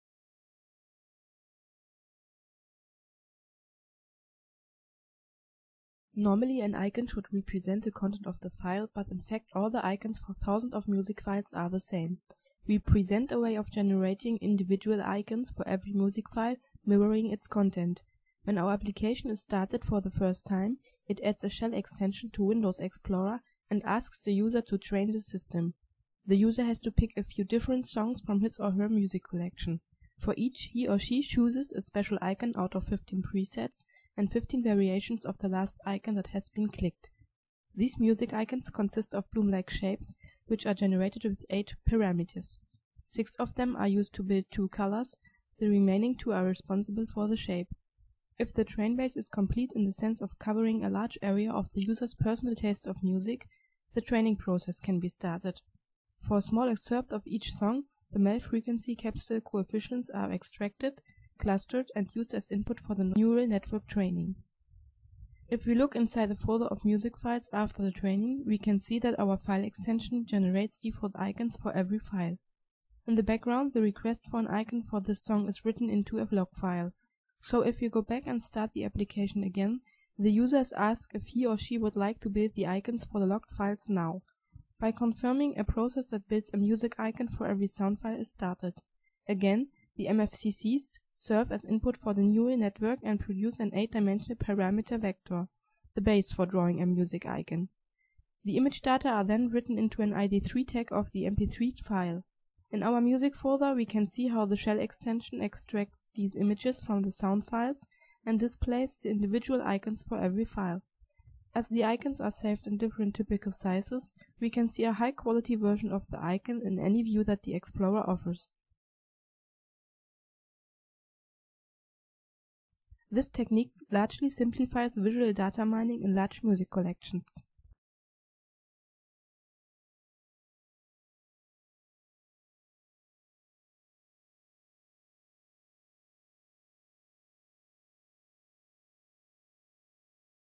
File Icons that Show the Content of Audio Files
It's hard to make sense of an MP3 collection when all files show the same icon. So let's automatically create a specific icon for each one. For the SIBGRAPI 2006 paper and the extended paper in Computers & Graphics 2008